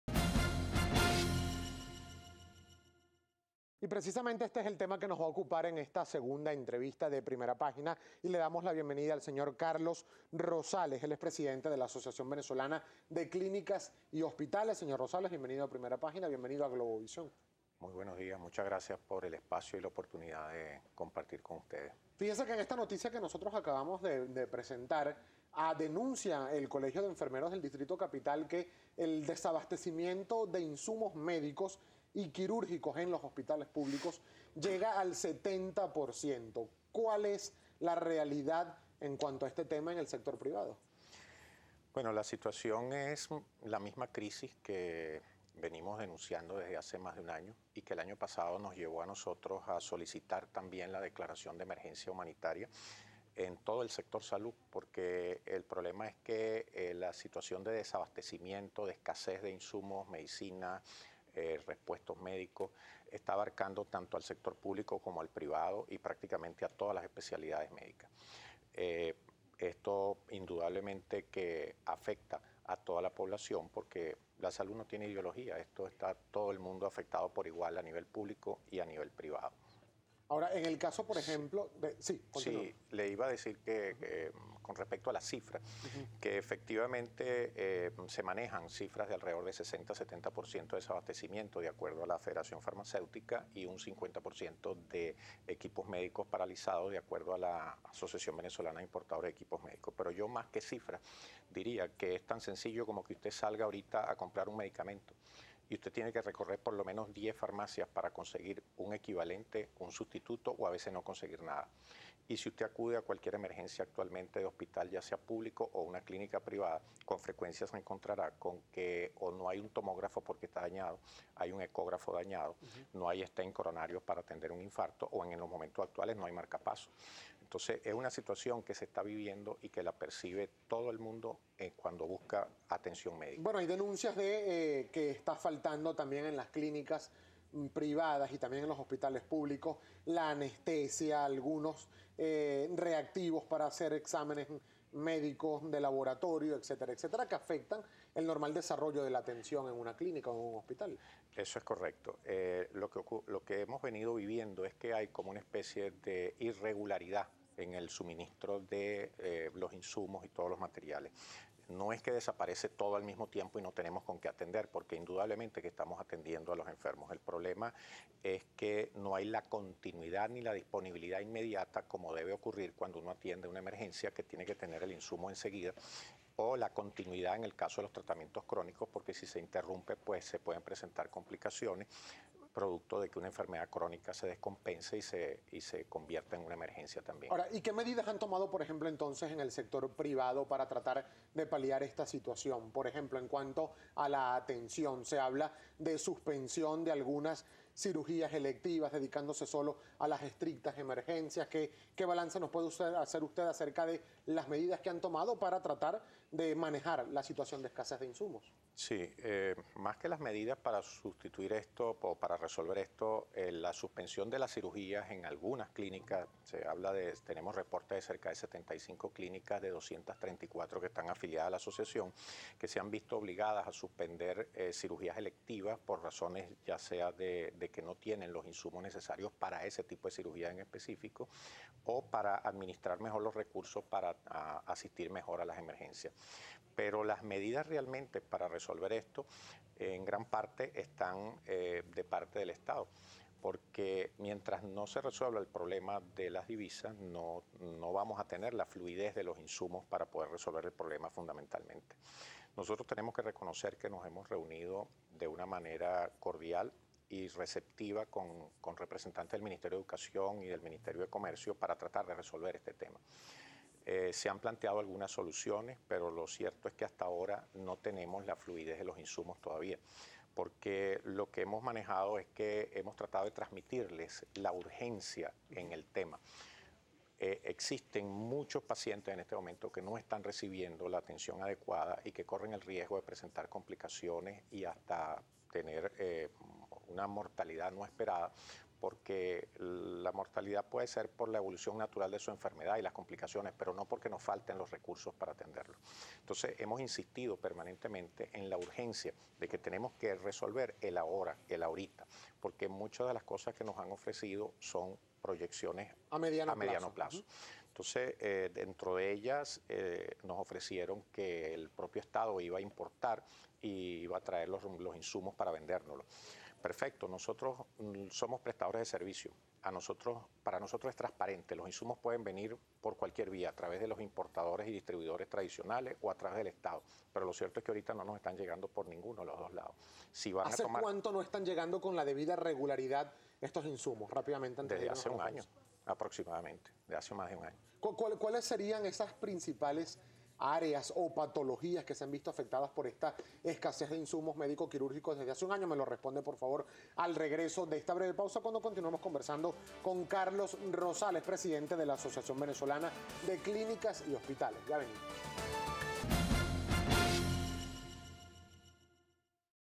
Denuncian suspensión de muchas cirugías electivas por falta de insumos
El presidente de la Asociación Venezolana de Clínicas y Hospitales, Carlos Rosales, indicó que en los centros de salud están atendiendo a los enfermos, pero no hay disponibilidad inmediata para las emergencias o continuidad para los tratamiento crónicos.